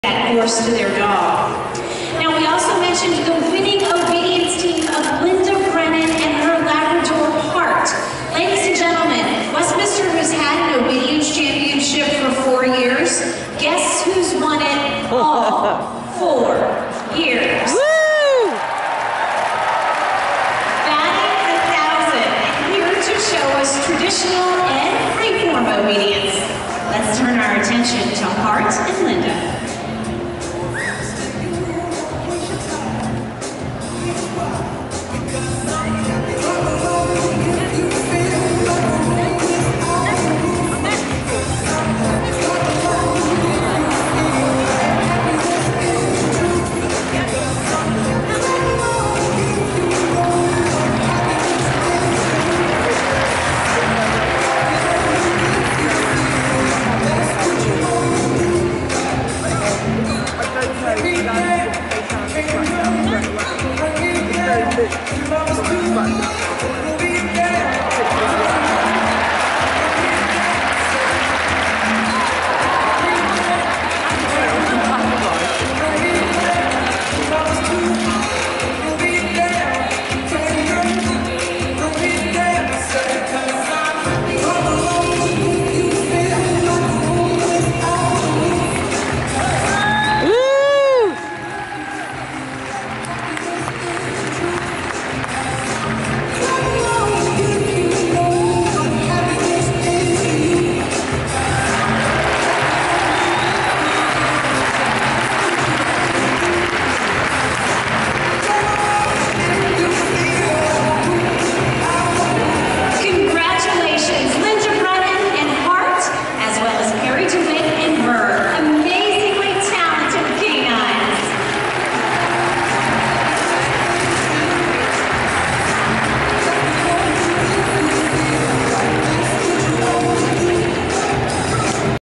Westminster 2019 - Heart's Demo at the Garden
Linda and Heart get to do a short demo from their winning freestyle routine at Madison Square Garden during the awards ceremony on Monday evening in between group judging.